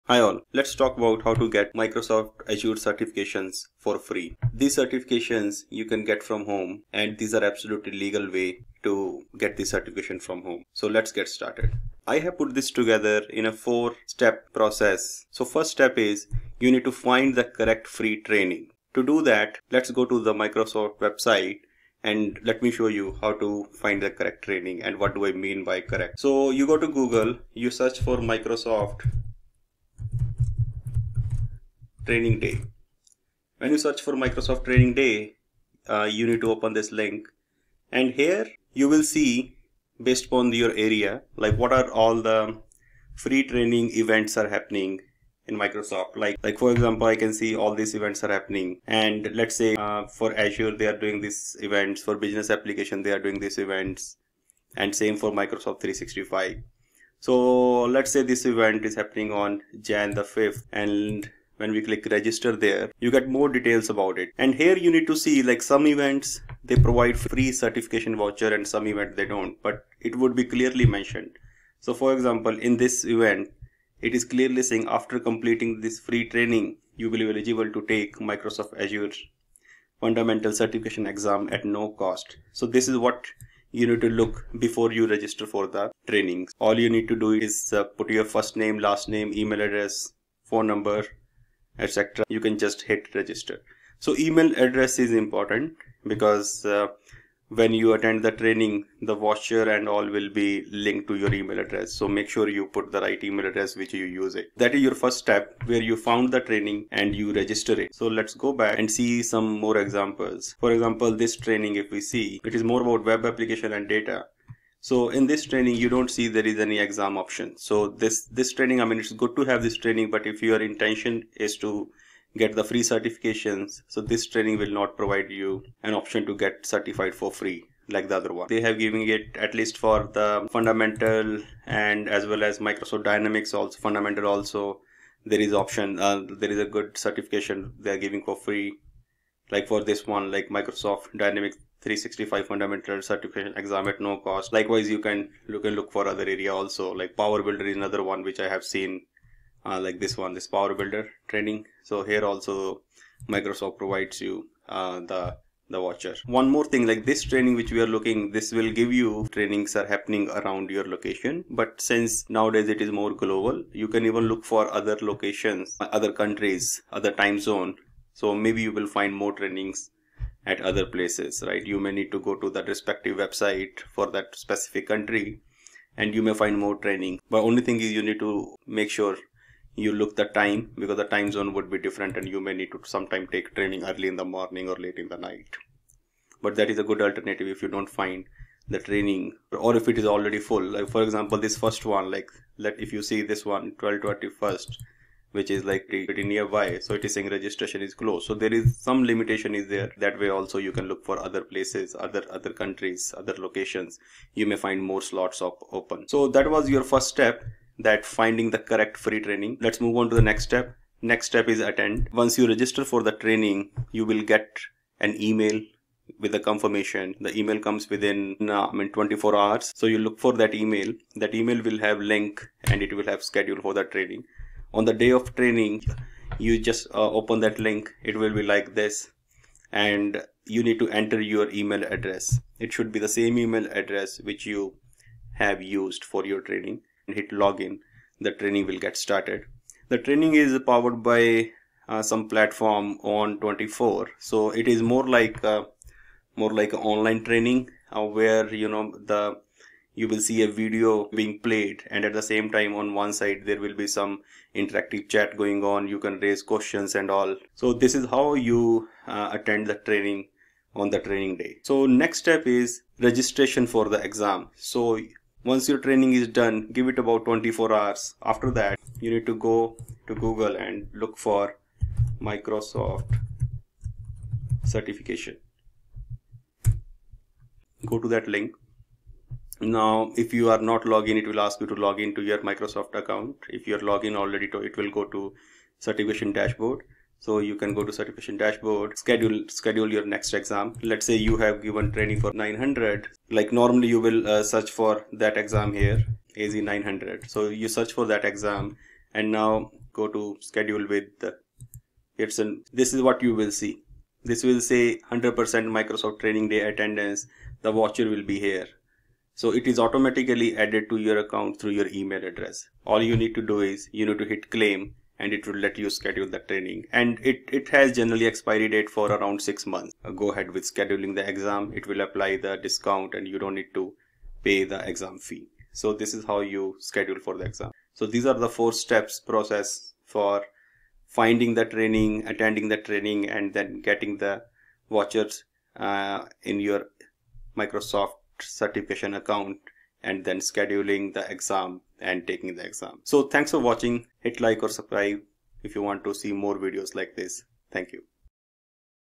FREE Microsoft AZURE certifications : a four-step guide!
This video demonstrates a step by step guide to register and legally get Microsoft certifications for Free.

If you are new to Cloud and want to attend Free training and get certified for Free, there are many providers encourage the learning by free offers. See this short video for FREE Microsoft AZURE training and getting FREE certifications vouchers : a four-step guide!

AZ-900
DP-900
AI-900
PL-900
MB-901